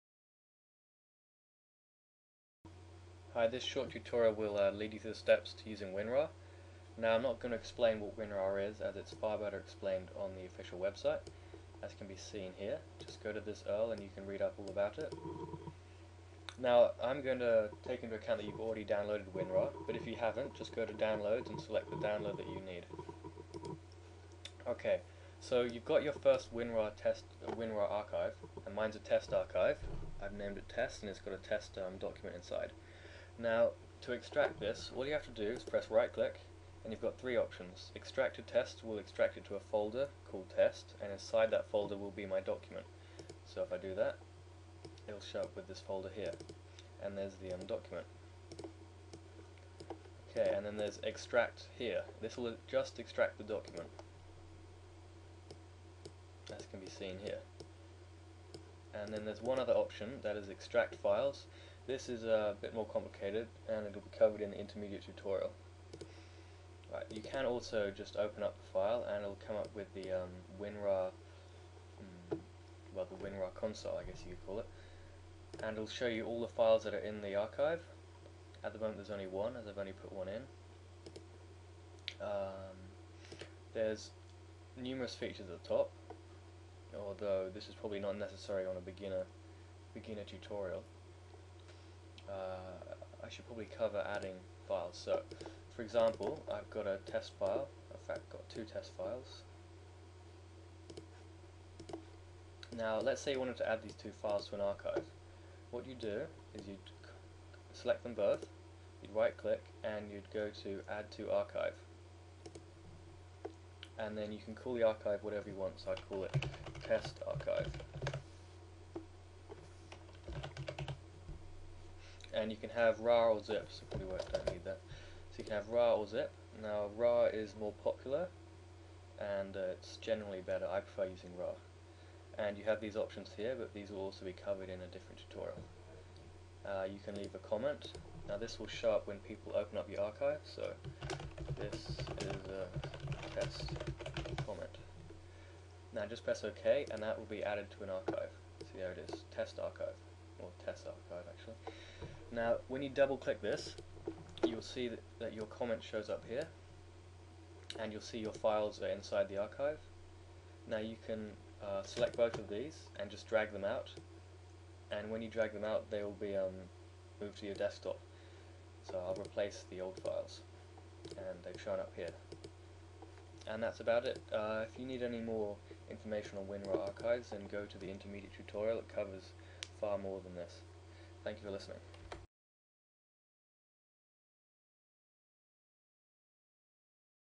How to use WinRAR - Beginner [With Voice]
This short tutorial will cover the basics of how to use WinRAR. Shows you how to extract files from an archive and also how to add files to an archive. For beginners

for more tutorials!